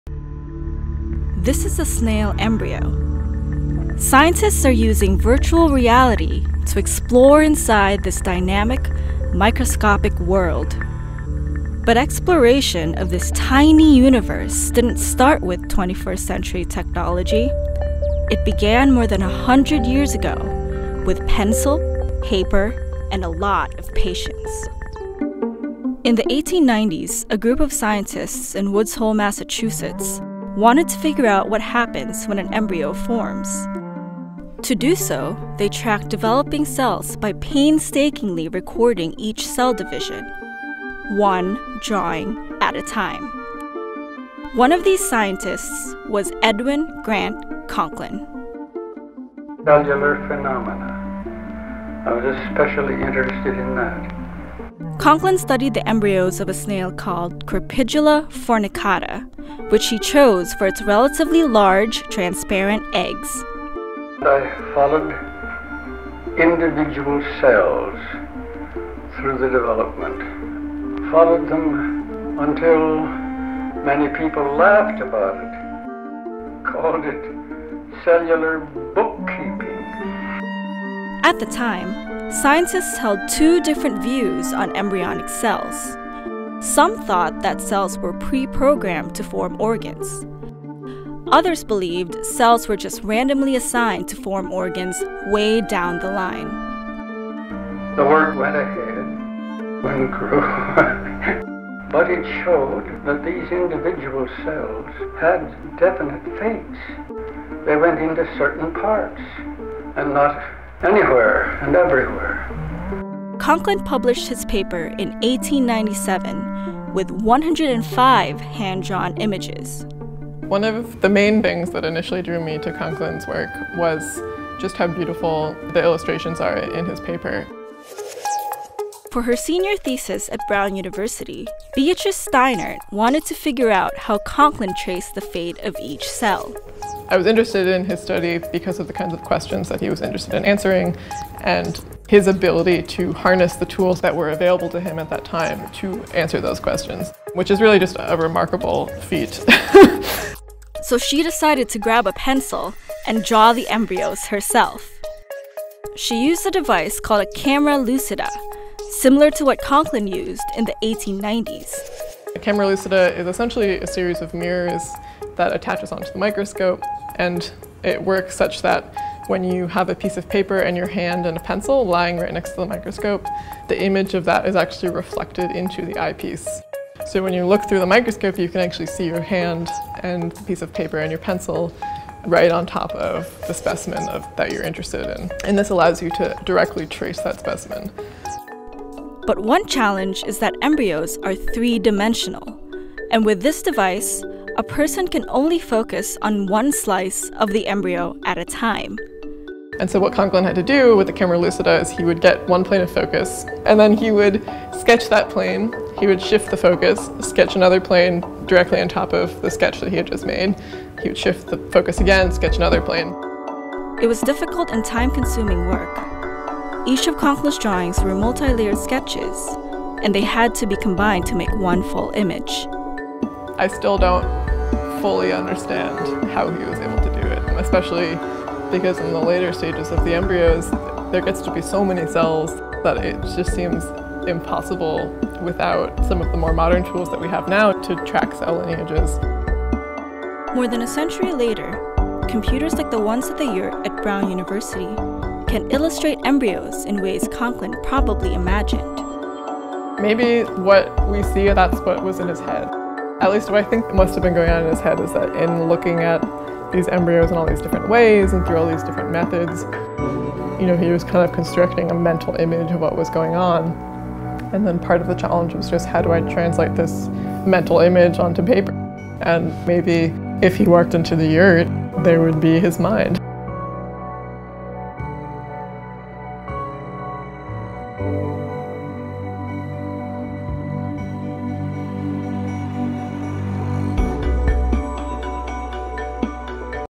Sketching the Beginnings of Life, One Cell at a Time
How did a scientist create incredibly detailed drawings of embryo development a century ago?